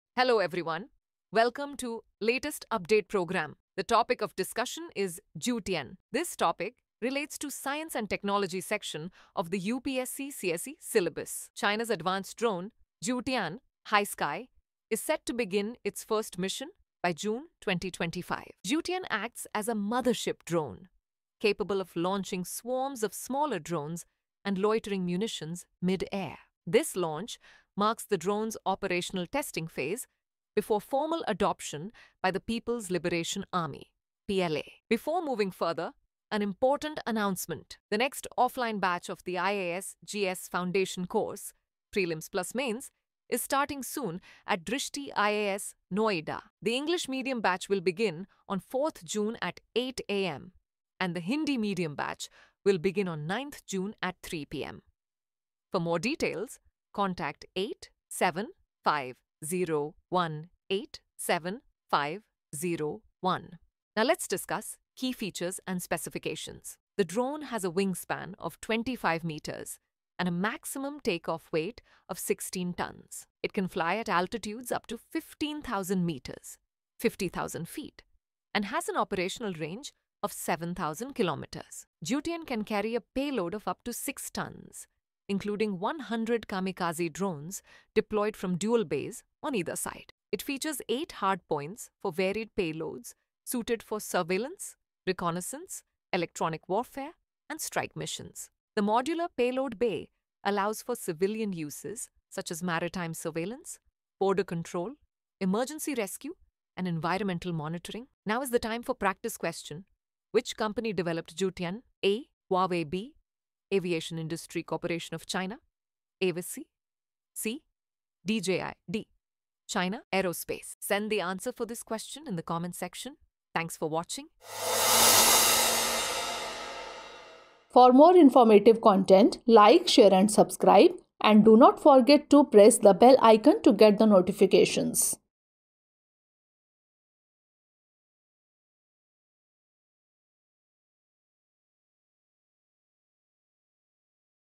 Jiu Tian | Mothership Drone | China | Latest Update | Drishti IAS English
China’s advanced drone, Jiu Tian (“High Sky”), is set to begin its first mission by June 2025. Jiu Tian acts as a “mothership” drone, capable of launching swarms of smaller drones and loitering munitions mid-air. This launch marks the drone’s operational testing phase before formal adoption by the People’s Liberation Army (PLA).

Dear Viewers,